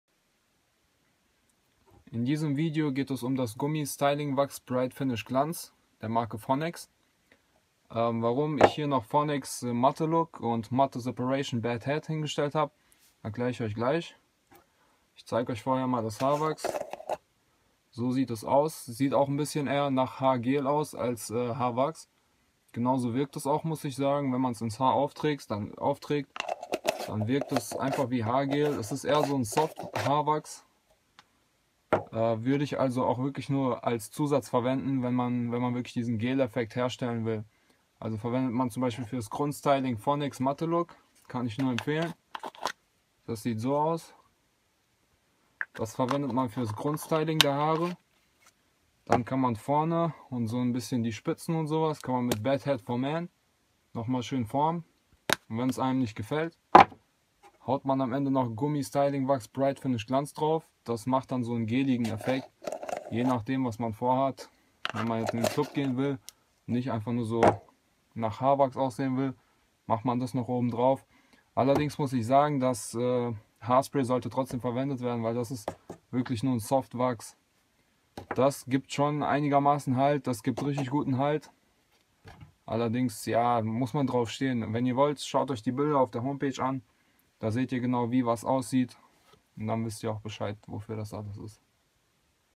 Haarwachs Test Fonex gummy styling Wax bright finish glanz - haarwachs-haargel-tests.de
Hier werden Haarwachse und Haargele getestet. Beim vorgestellten Haarwachs handelt es sich um eine Art Spezial-Wachs um den Haaren eine Art Gel-Look zu verpassen, nach dem Styling mit Haarwachs.

Haarspray ist empfehlenswert zur Festigung der Haare (Verändert den Look kaum). Ein gutes ist

Viel Spaß